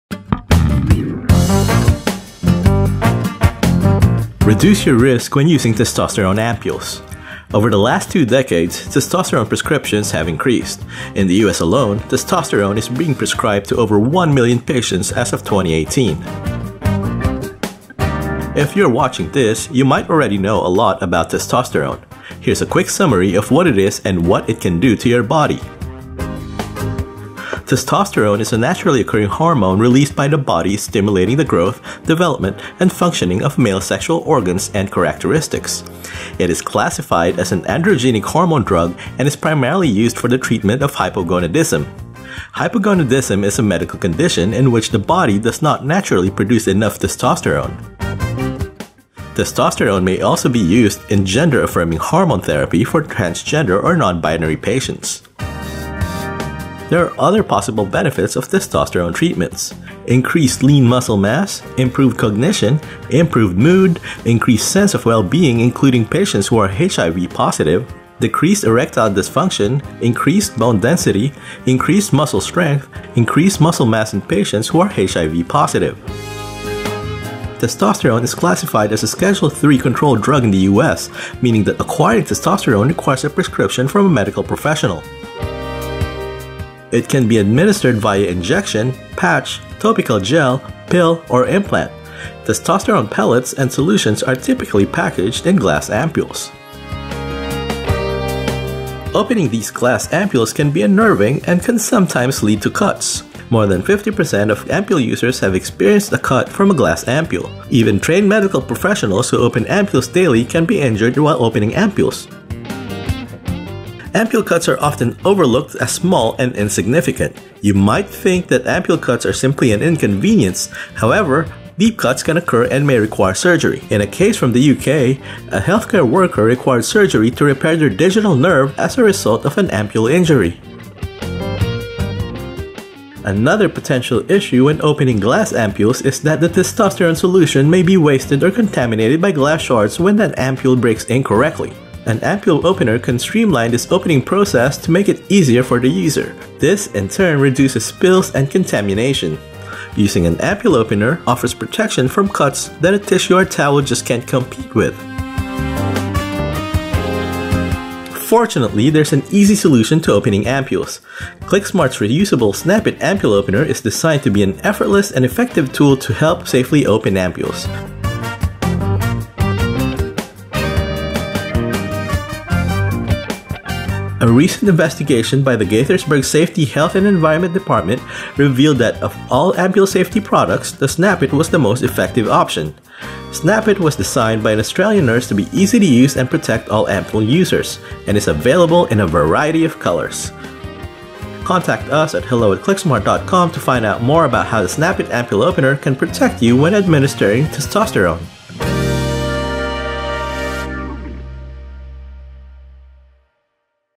How to Open Testosterone Ampoules EASILY and SAFELY.
Testosterone pellets and solutions are typically packaged in glass ampoules. Opening these glass ampoules can be unnerving and can sometimes lead to cuts.
In a study regarding the incidence of ampoule injuries, more than 50% of ampoule users have experienced a cut from a glass ampoule. An ampoule opener can streamline this opening process to make it easier for the user. This in turn reduces spills and contamination.
Fortunately there’s an easy solution to opening ampoules. Qlicksmart’s reusable SnapIT ampoule opener is designed to be an effortless and effective tool to help safely open ampoules. A recent investigation by the Gaithersburg Safety, Health, and Environment (GB SHE) department revealed that, of all the ampoule safety products, the SnapIT was the most effective option.